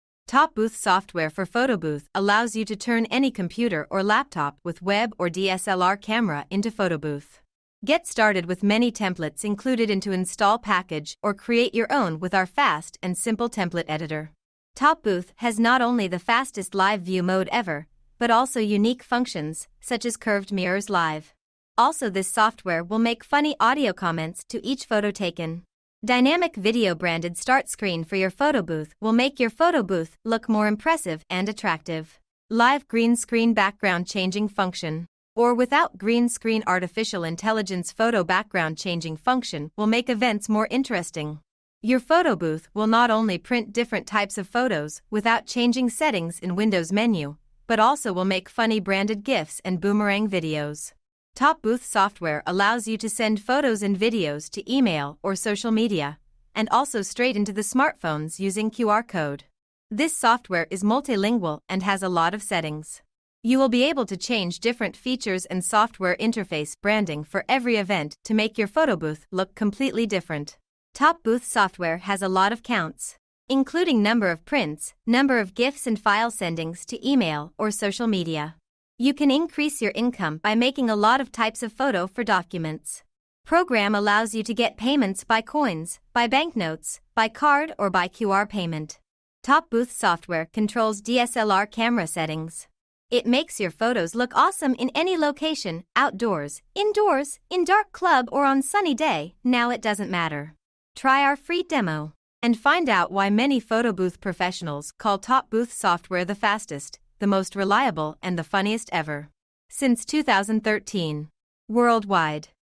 TopBooth - PhotoBooth software with unique functions
TopBooth - The funniest PhotoBooth software

TopBooth software for photobooth allows you to turn any computer or laptop with web or DSLR camera into photobooth

Get started with many templates included into install package or create your own with our fast and simple Template editor

TopBooth  - has not only the fastest Live View mode ever, but also unique functions, such as Curved mirrors live. Also this software will maky funny audio comments to each photo taken. Dynamic video branded start screen for your photobooth will make your photo booth look more impressive and attractive. And also many other features you'll defenetely like!

Live  green screen background changing function -. or  WITHOUT green screen artifitial intelligence photo background changing function  will make events more interesting!

Your photo booth will not only print different types of photos -  without  changing settings in windows menu, - but also will make funny branded GIFs and Boomerang videos

TopBooth software allows you to send photos and videos to e-mail or social media, and also straight into the smartphones using QR code
This software is multilingual and has a lot of settings. You will be able to change different features and software interface branding for every event to make your photobooth look completely different

TopBooth software has counts number of prints, number of GIFs and files sending etc. It has automatic photo and files sending function and many other features

You can incrtease your income by making a lot of types of photo for documents. Programm allows you to get payments by coins, by bancnotes,  by card or by QR payment.

Try our FREE demo -. and Find out why many PhotoBooth professionals call TopBooth software the fastest, the most reliable and the funniest ever. Since 2013. Worldwide.

TopBooth software controls DSLR camera settings and makes you photos look awesome in any location: outdoors, indoors, in dark club or on sunny day